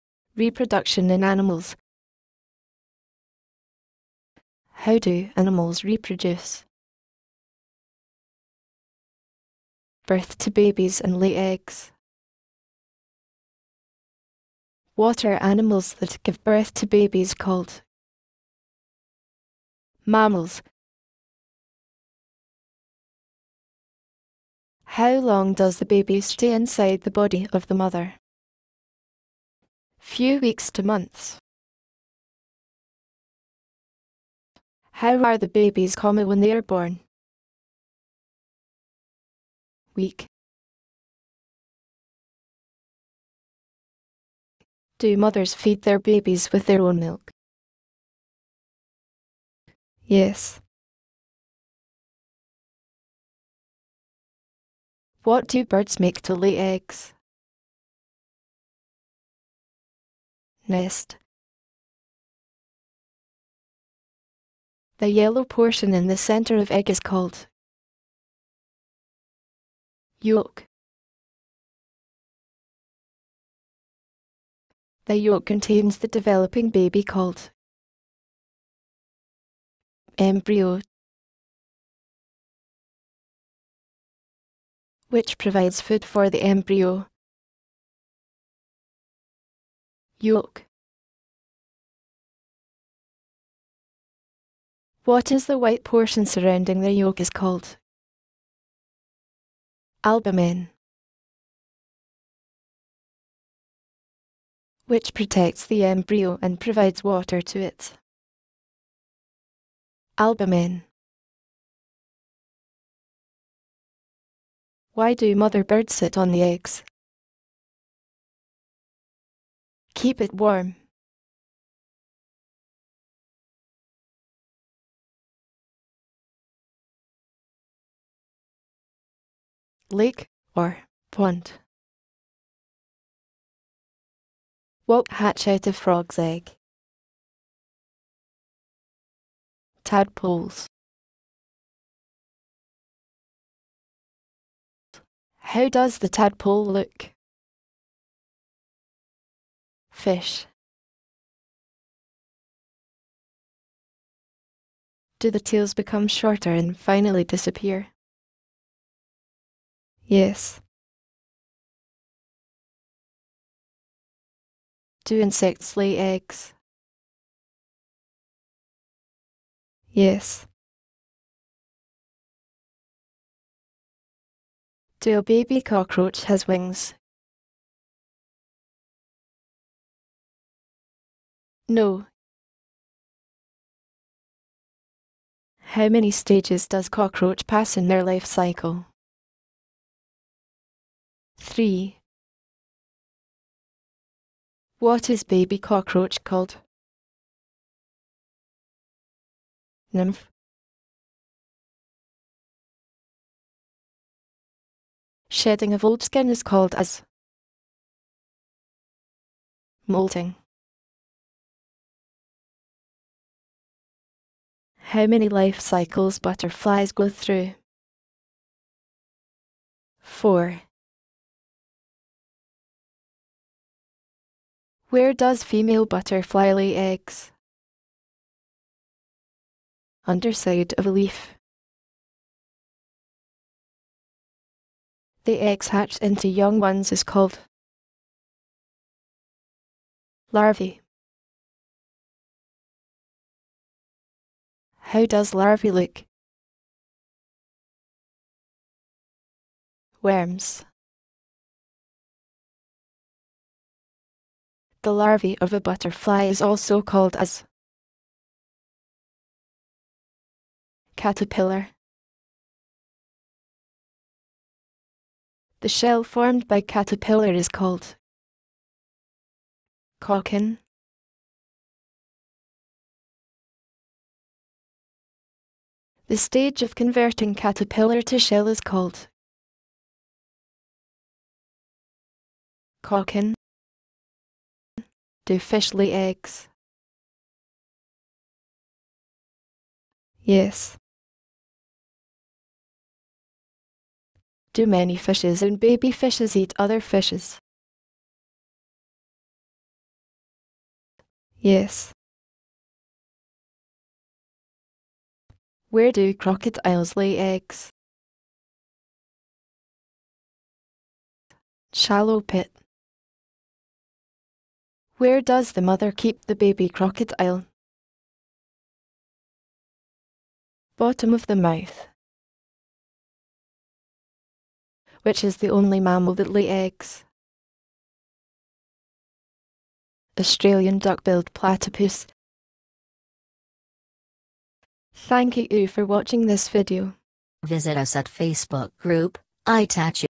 Reproduction in Animals-Class 4
This video describes about reproduction in animals.This video covers class 4  syllabus.The video shows mammals and animal that lay eggs.What is a yolk?what is an embryo?what is an albumen? the life cycle of frog and butterfly are given..What are tadpoles and nymph are given here...